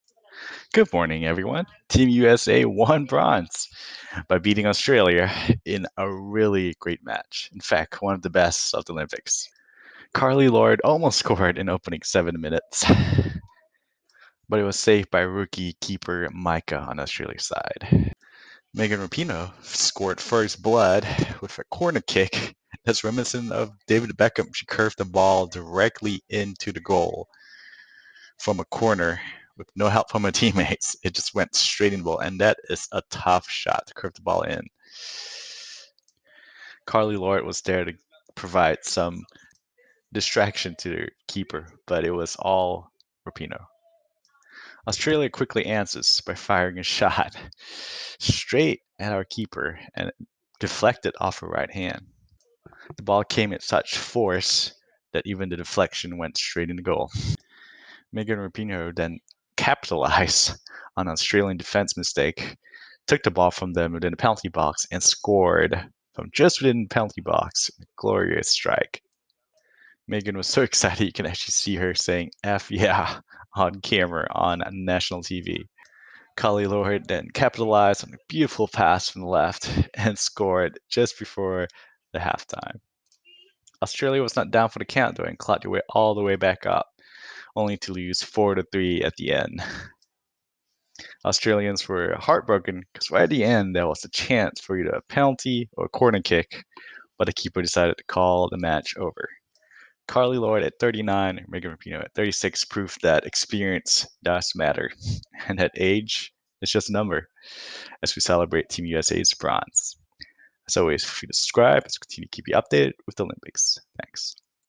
USA Wins Bronze after Beating Australia 4-3 - Women's Soccer - 2021 Tokyo Summer Olympic Games
It had to be them. Playing in what could have been the final Olympic match of their career, U.S. women’s national team veterans Megan Rapinoe and Carli Lloyd each scored two goals as the USA claimed the women’s soccer bronze medal, 4-3 over Australia. It was a vintage high-pressure, high-scoring U.S. performance and one that was missing for most of these Olympics.

The 36-year-old Rapinoe scored the Americans’ first two goals in spectacular fashion: her opening strike in the 8th minute was fittingly an 'olimpico' goal, as she scored directly off a corner kick. Her second was equally spectacular on a volley into the top corner after a defensive clearance fell perfectly to her in the box.

The 39-year-old Lloyd became the USA’s all-time leading scorer at the Olympics with two goals, one on either side of halftime, and both were classic forward finishes. The first was a spectacular diagonal shot inside the far post, and the second was a classy nutmeg of the goalkeeper.